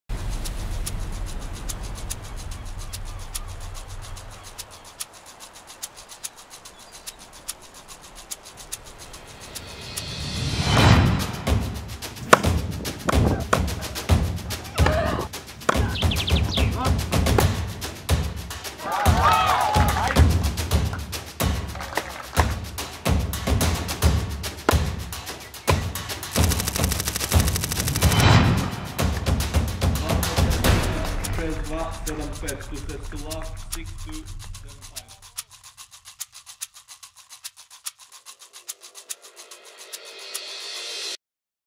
WTA Makarska Open 2022 - After tournament video (short version)
Short video showing WTA Makarska Open 2022 tournament highlights.